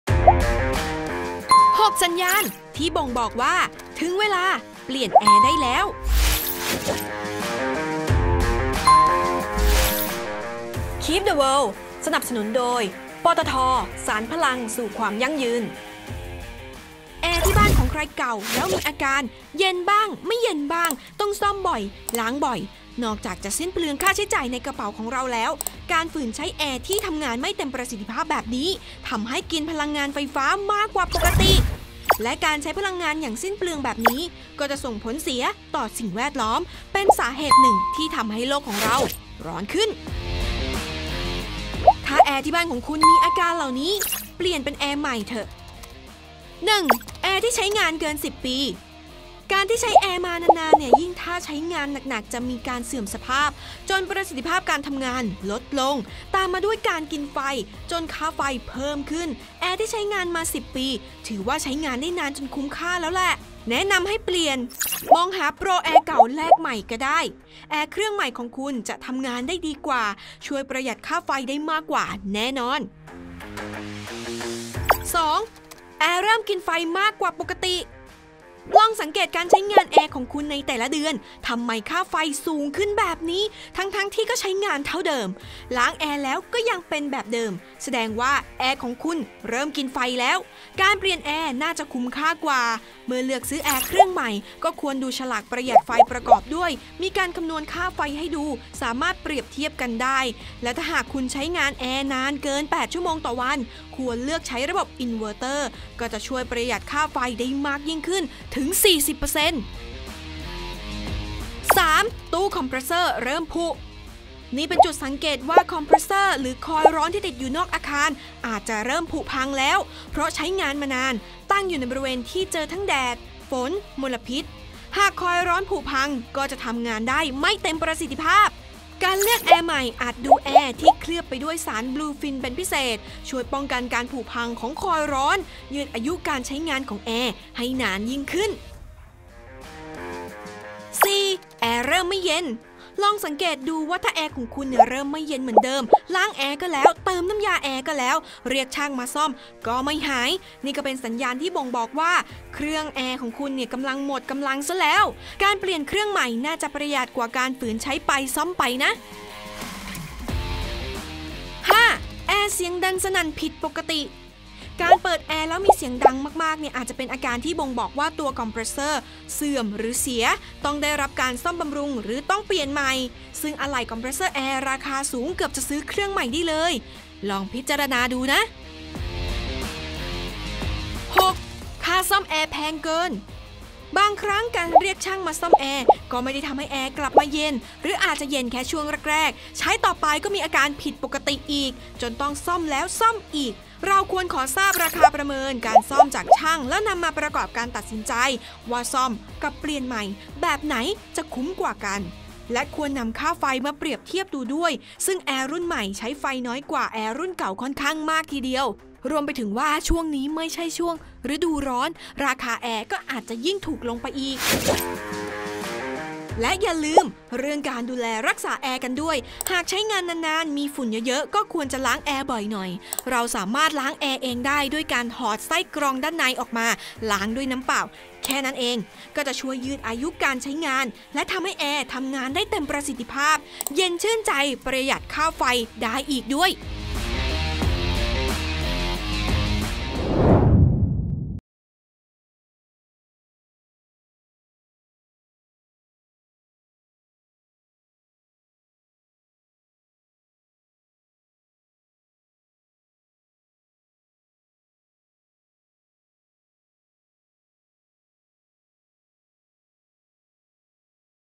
6 สัญญาณเตือน ควรเปลี่ยนแอร์ใหม่ได้แล้ว | KEEP THE WORLD | SPRiNG
ช่วงนี้ค่าไฟทำไมพุ่งขึ้นแบบนี้ สาเหตุอาจเป็นที่แอร์ของคุณใช้งานมาเยอะหรือเปล่า ลองมาสำรวจกันหน่อยว่า สัณณาณแบบไหนที่แอร์ของคุณกำลังกินไฟและควรเปลี่ยนแอร์ใหม่ได้แล้ว